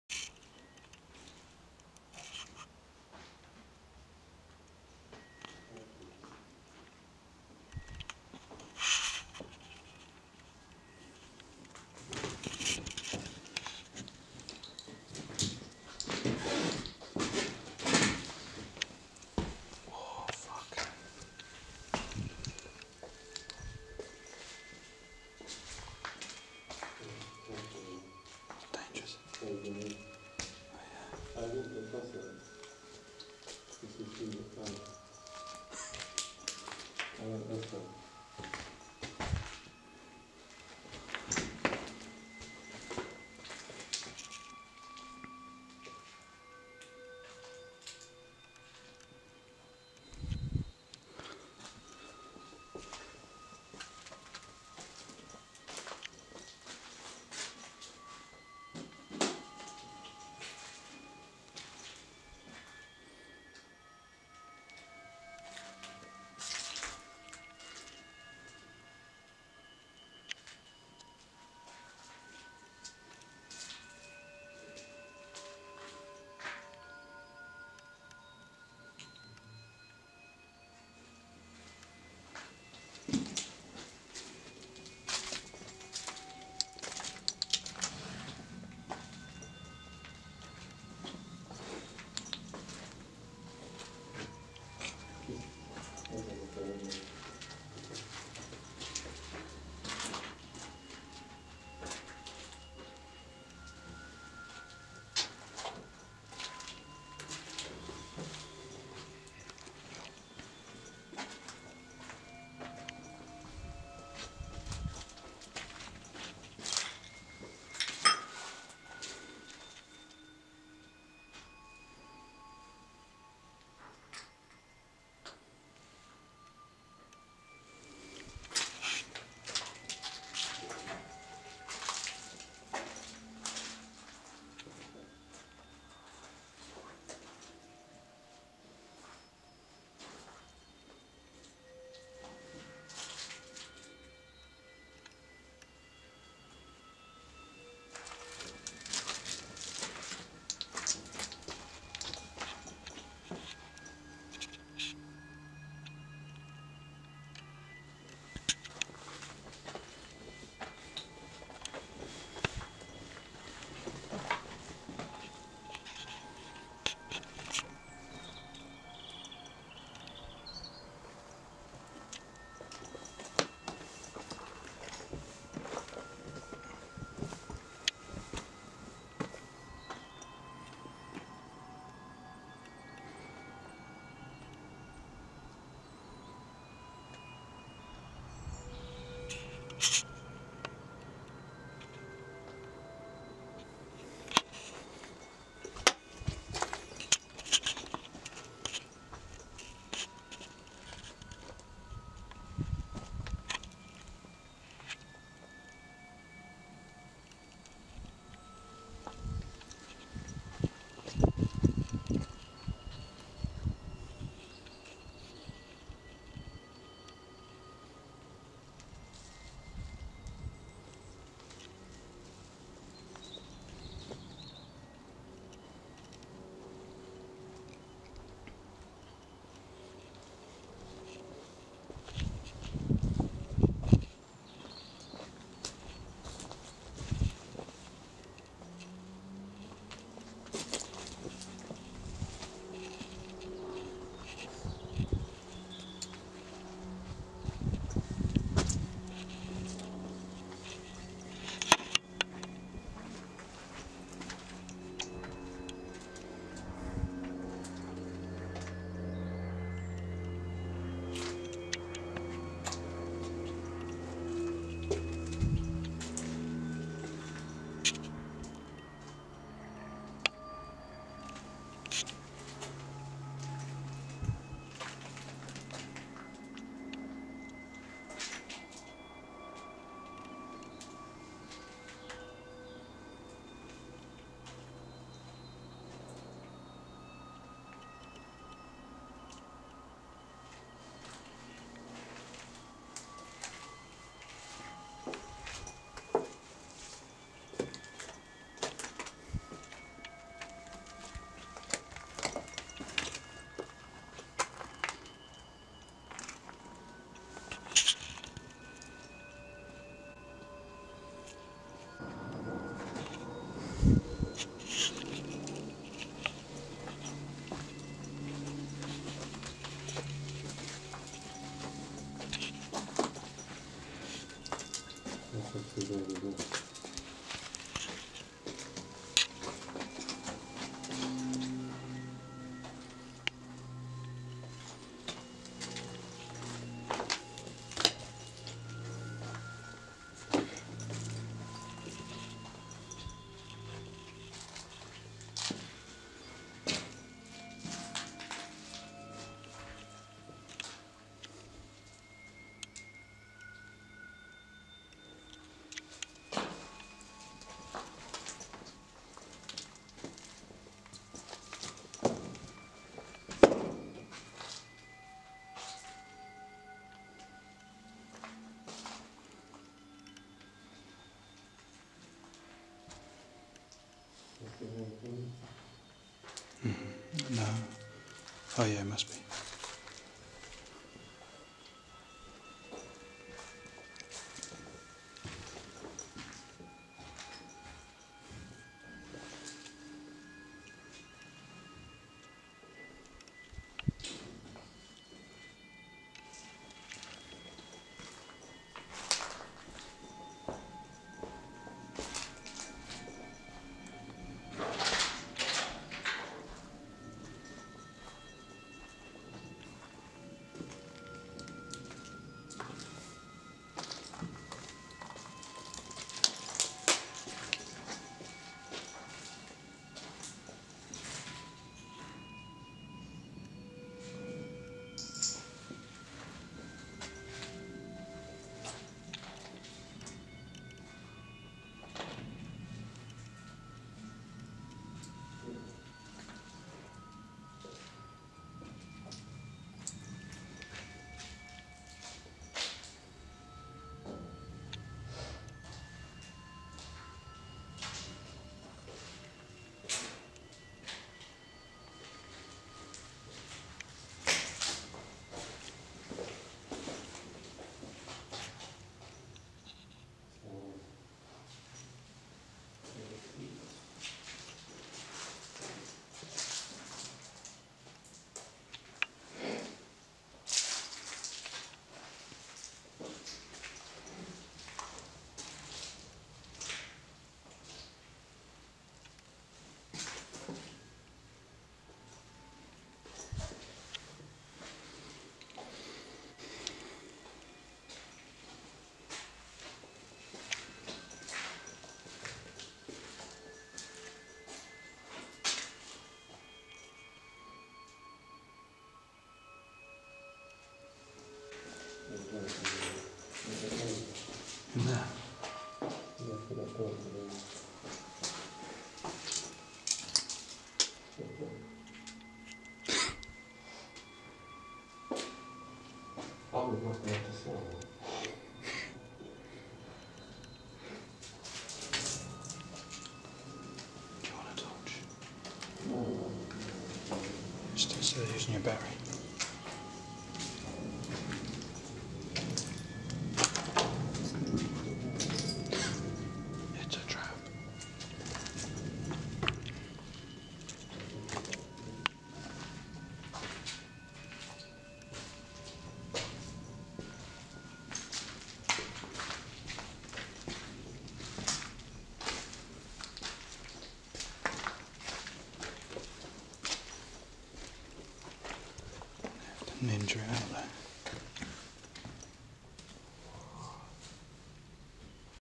Explore Abandoned Mental Hospital (SCARY!) - Building 2
Harperbury Hospital, Hertfordshire, UK

My second explore of Harperbury Hospital with a fellow urbexer. This building was similar to Building 1, however, we discovered a bath!
Again, security with dogs were patrolling the grounds as we were about to exit (not on film) which meant we were trapped for some time. Another chilling, but exciting explore!

The building appears to be a residential building on the site, perhaps a dormitory housing many patients. The building contains a large living room downstairs and a kitchen. Upstairs contained two large bedrooms with bathroom facilities.